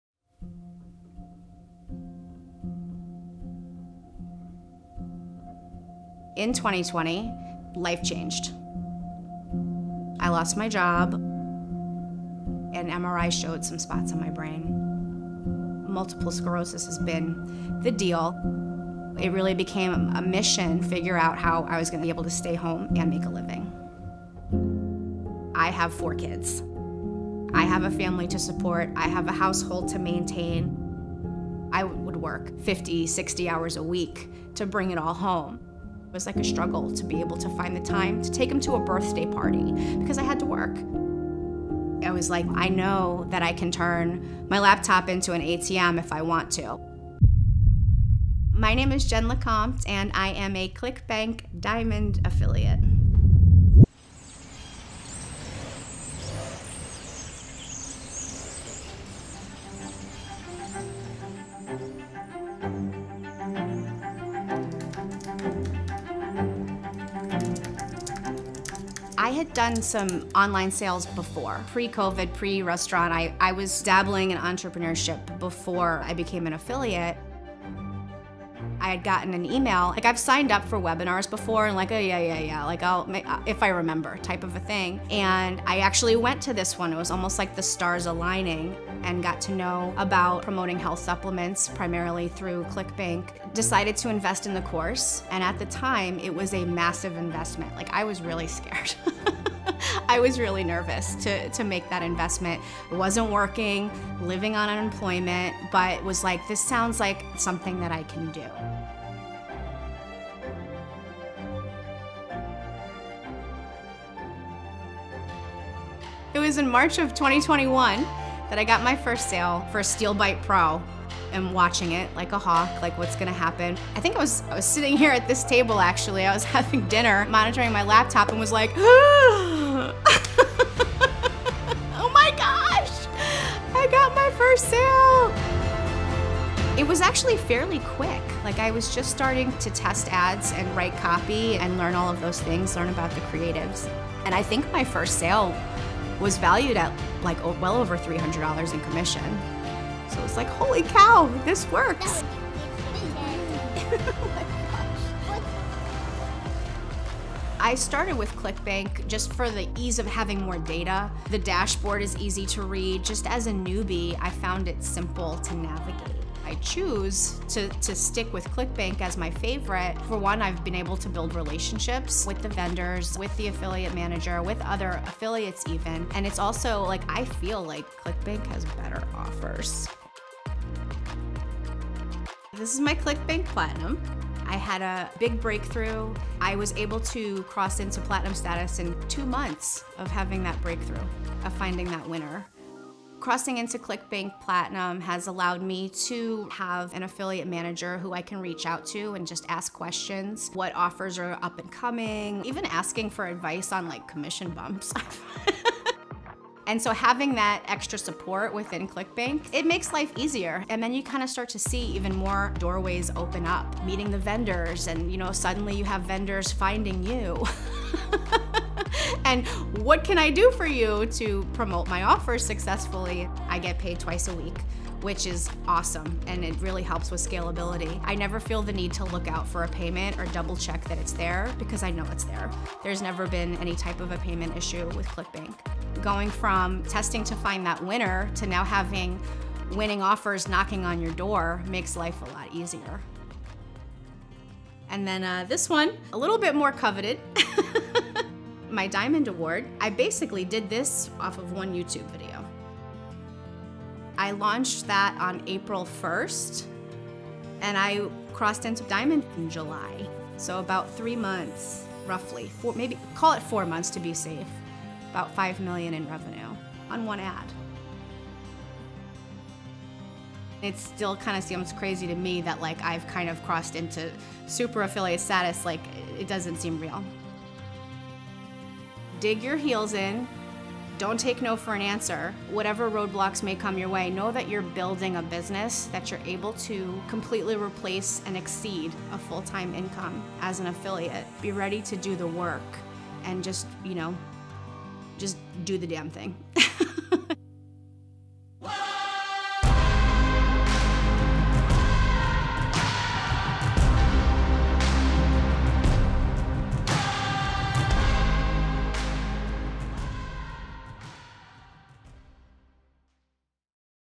Mother of 4 Turned $5M+ ClickBank Super Affiliate in Under a Year
Jen LeCompte has such a powerful ClickBank affiliate journey to share. In 2020, Jen lost her job. She was diagnosed with MS. AND she had 4 kids to care for.

With little options available, Jen turned to online education about affiliate marketing. After learning some tips and tricks, she stumbled upon ClickBank and decided to give affiliate marketing a shot.

She made some creative and promoted her first product. And then she waited. What happened next would change her life forever.

We hope you enjoy Jen's journey from waitress to $5M+ ClickBank super-affiliate in under a year!

This is an amazing story of how moms can make money from home.

🔥 LINKS 🔥